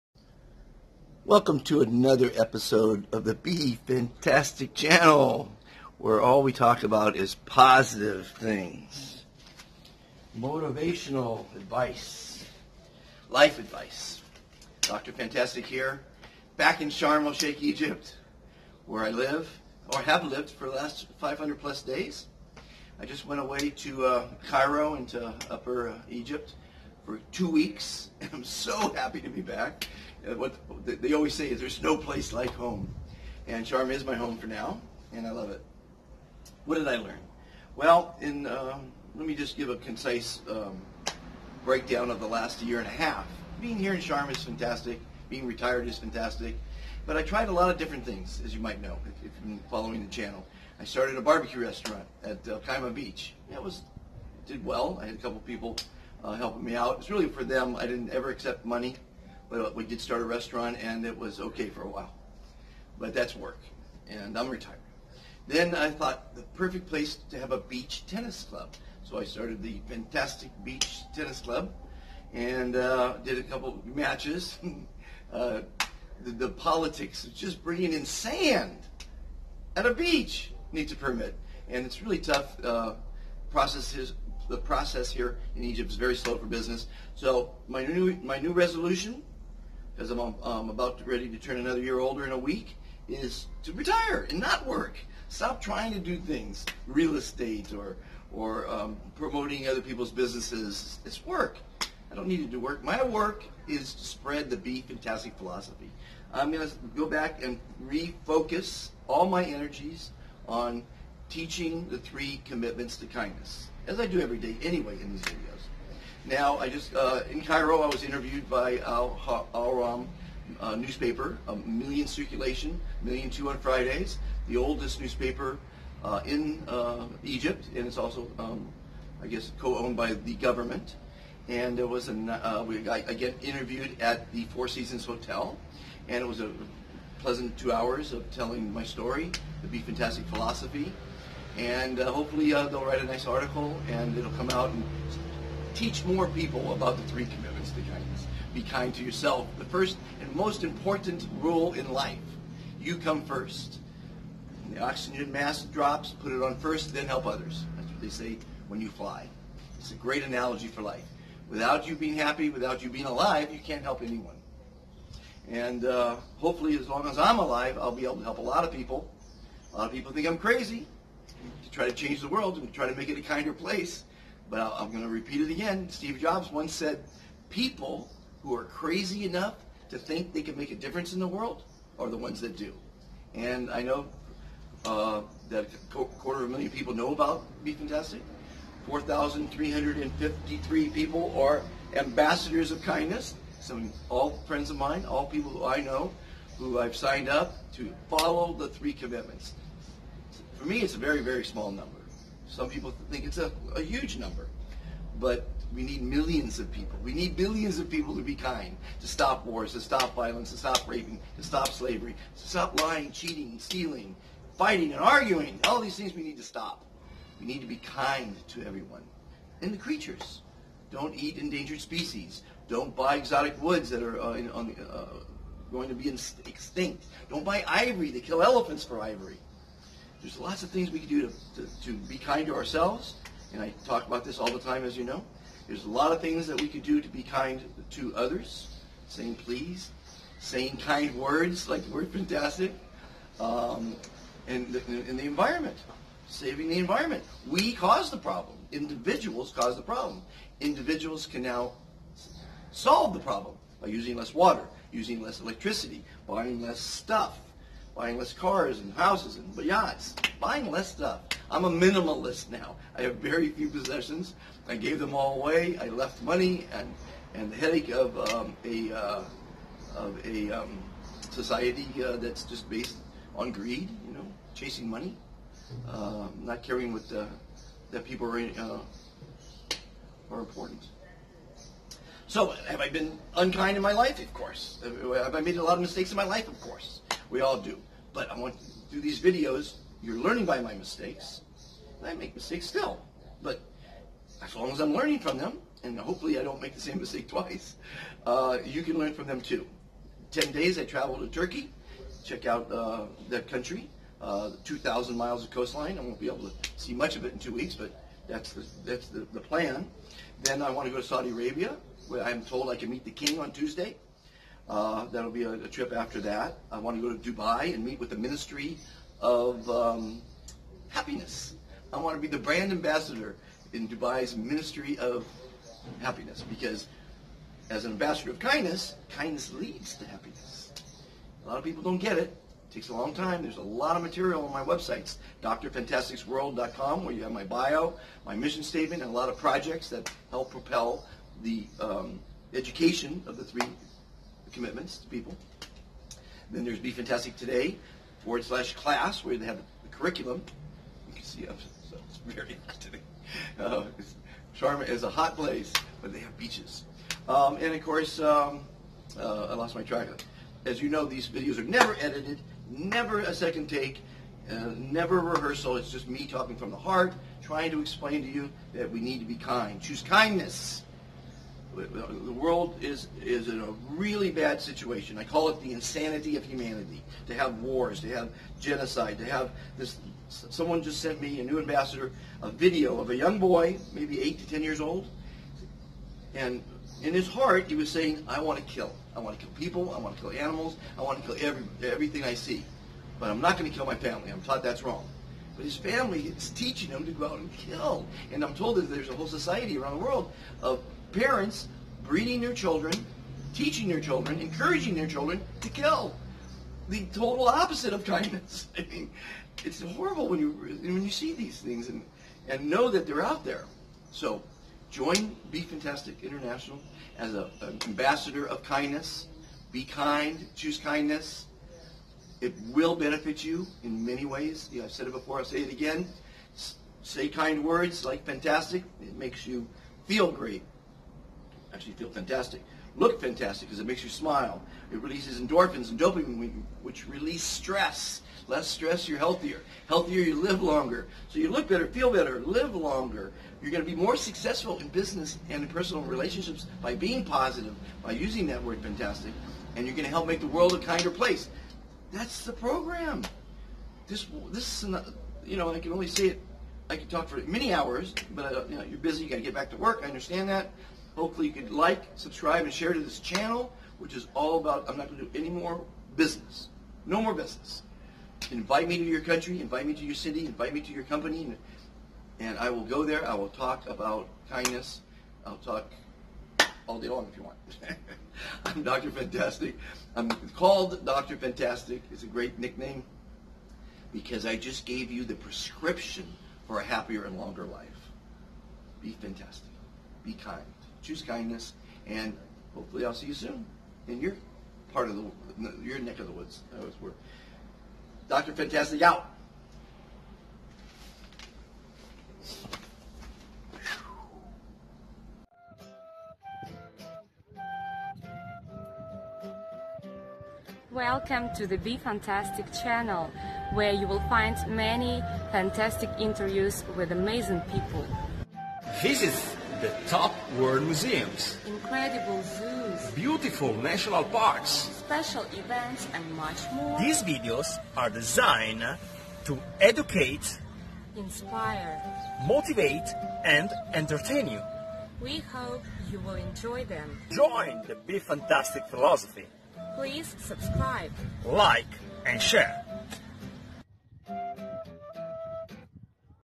Back in Paradise
There is no place like home is the saying, there is no place like Sharm el Sheikh Egypt. Come visit Dr. Fantastic!